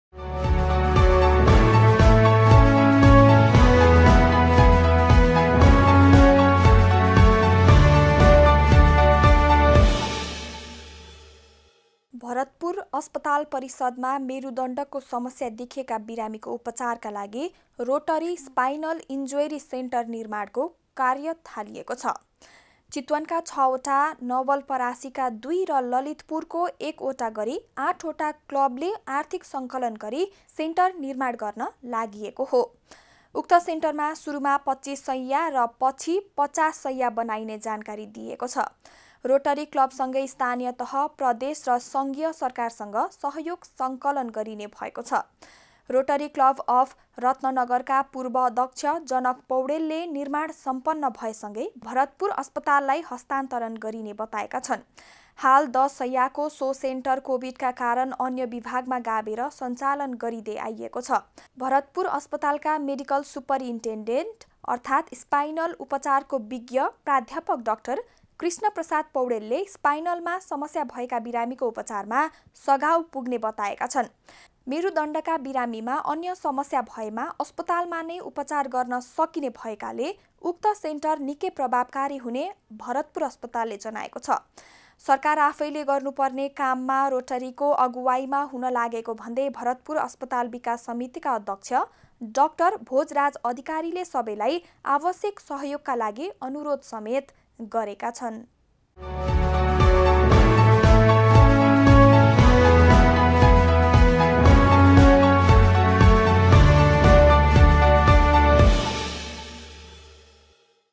भरतपुर अस्पतालमा स्पाइनल इन्जुरी सेन्टर निर्माणको कार्य सुरु
Health television Nepal present :- भरतपुर अस्पताल परिसरमा मेरुदण्डको समस्या देखिएका बिरामीको उपचारका लागि रोटरी स्पाइनल इन्जुरी सेन्टर निर्माणको काम थालिएको छ । चितवनका ६, नवलपरासीका दुई र ललितपुरको एक गरी ८ वटा क्लबले आर्थिक सङ्कलन गरी सेन्टर निर्माण गर्न लागेका हुन । उक्त सेन्टरमा सुरुमा २५ शय्या र पछि ५० शय्या बनाइने जानकारी दिइएको छ ।